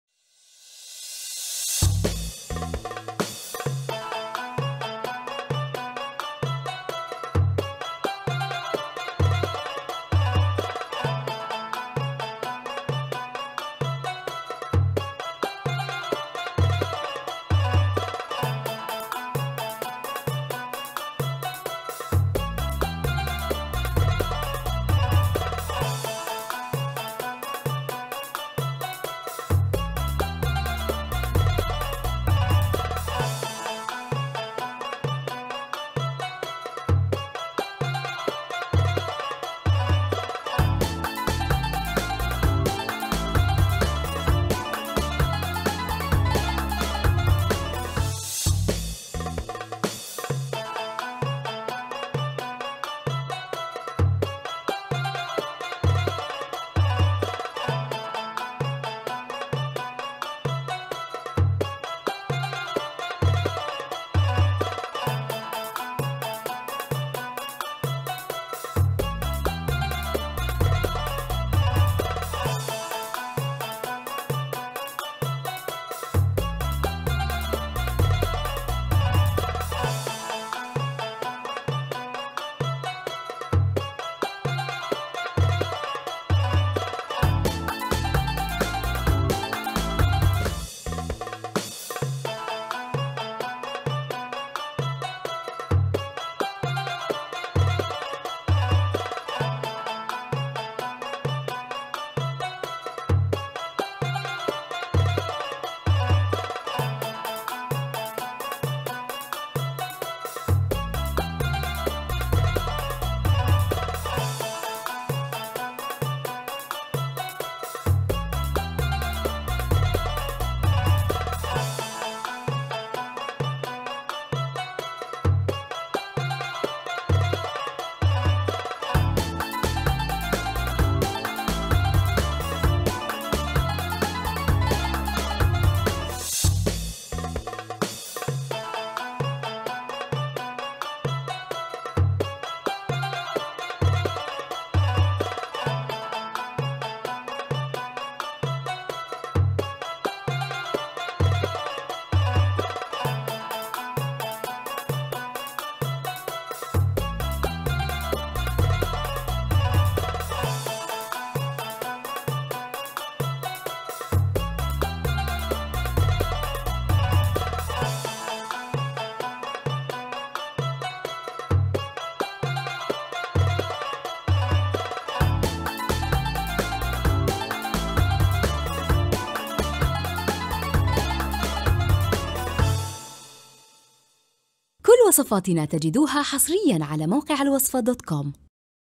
وصفات ايس كريم فى رمضان
أرز باللبن بالأيس كريم، أيس كريم بالبسكويت و البندق، أيس كريم كابيتشينو، أيس كريم الكانتلوب، سبليت الخوخ المشوي بالايس كريم وصفات مختلفة للايس كريم